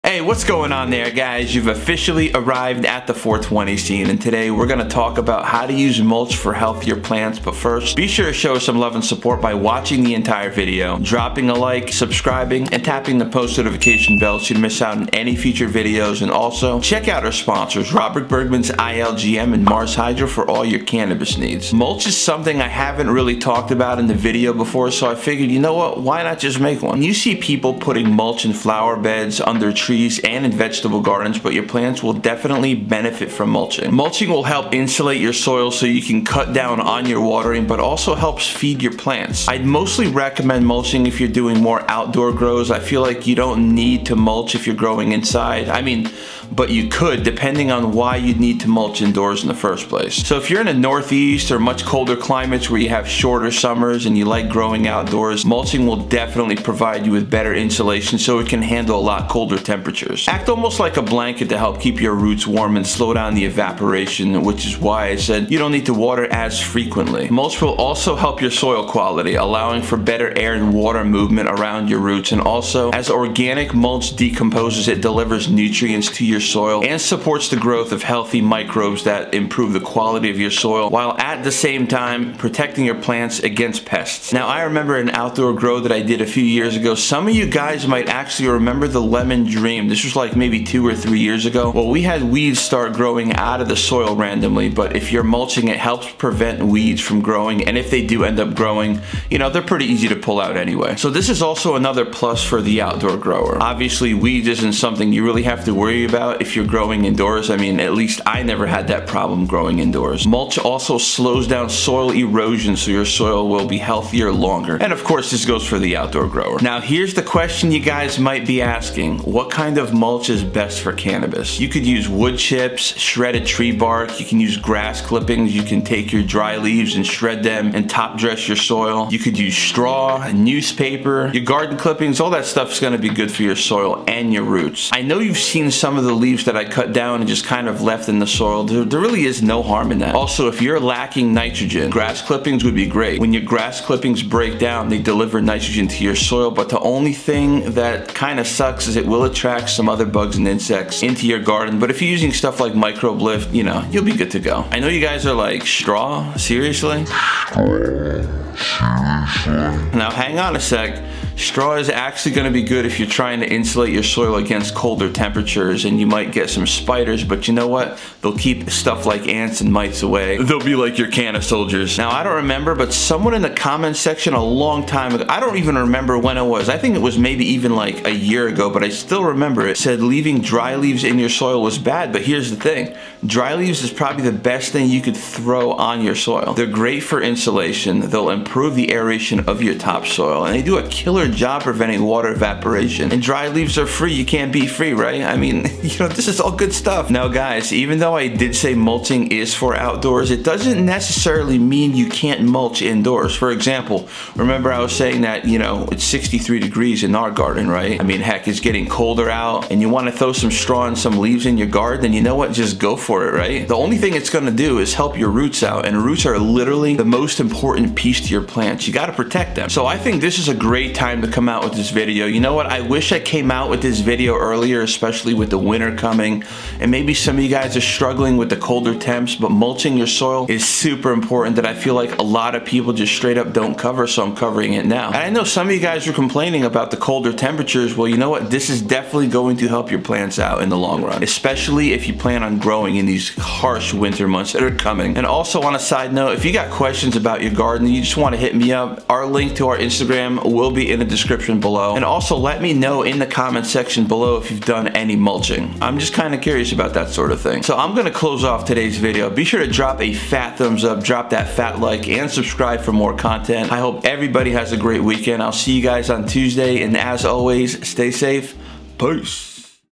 HOW TO USE MULCH FOR HEALTHIER PLANTS!
Get healthier cannabis plants with stuff like straw mulch, keep the moist content high, less water evaporation, keeping the ground free from direct sunlight keeping the microbial life thriving and going strong and these tips make all difference in the world come harvest time.

Banner Artwork: IG: alysha_kitty17

Camera I use

Mic I use

Computer I use

Interface I use

Fox Farm Ocean Forest